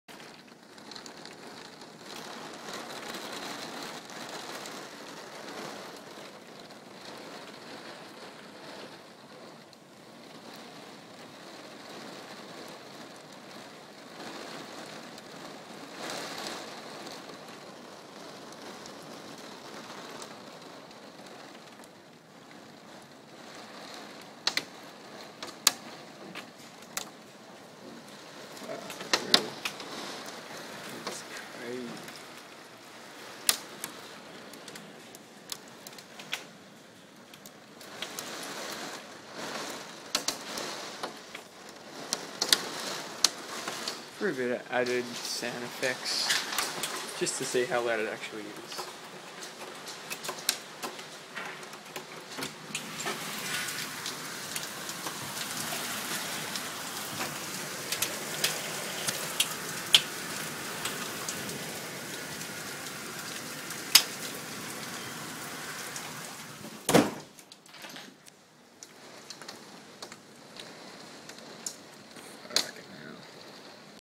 melbourne storm = crazy shit
about 20 minutes after this video melbournes biggest storm in year, soon i will put up a pt.2 showing the storm,